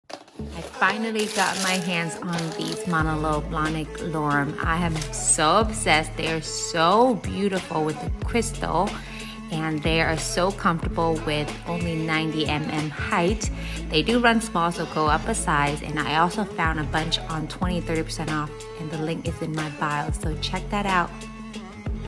Manolo Blahnik Lurum unboxing & try-on haul
Finally got my hands on these gorgeous yellow Manolo Blahnik Lurum. I’ve been wanting them for a while. These are 90mm (3.5 inches) tall so they’re quite easy and comfortable to wear. They do run small so go up 1/2 to a full size. I’m normally in size 35 but these are in size 36.

Yellow Manolo Blahnik Lurum

Shop my posts